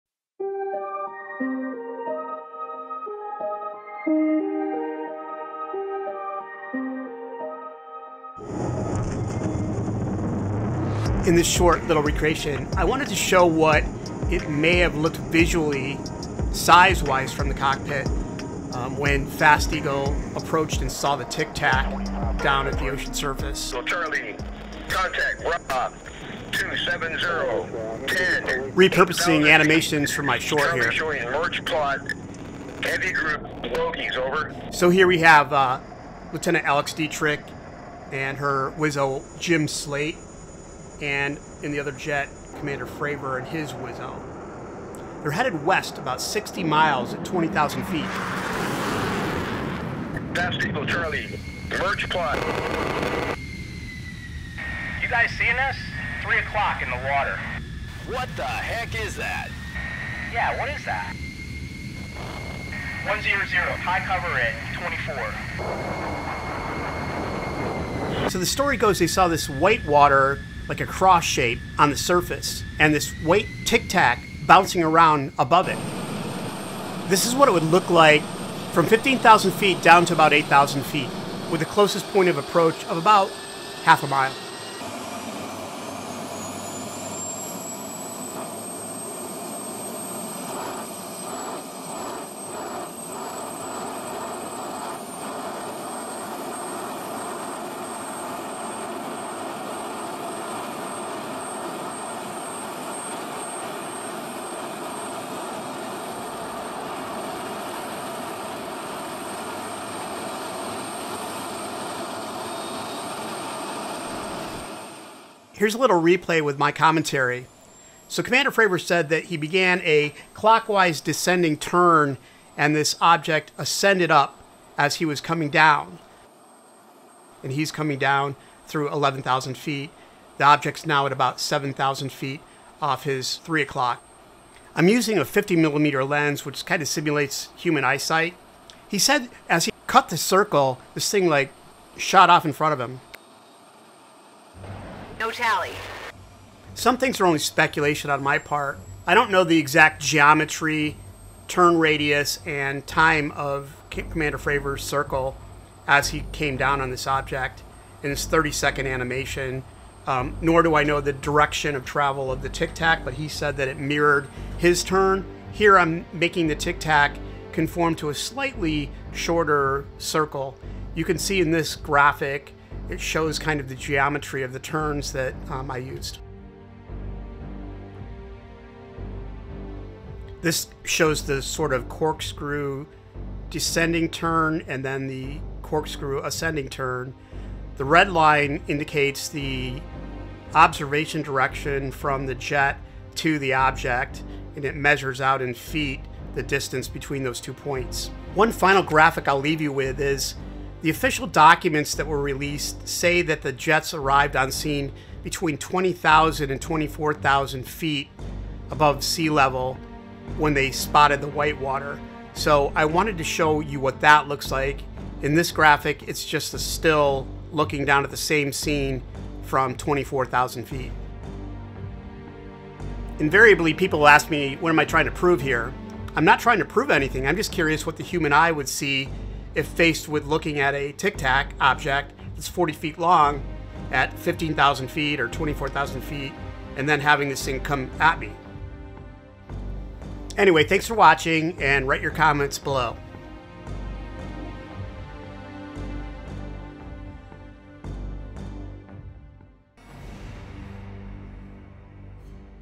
Nimitz Encounters Visual Simulation of DIstances
In this short, I used the physical modelling software Cinema 4D to re-create the exact reported distances from Cdr. David Fravor's jet to the ocean disturbance and the tic tac object. Then I created a sim of a descending clockwise turn down to about 8K, while the Tic Tac ascends to co-altitude.  The closest approach is about 1/2 mile. The ocean disturbance is 135 long and the tic tac is 40 long. The camera settings are a 50mm lens and 35mm sensor.  This should approximate what it looked like that day in November 14, 2004.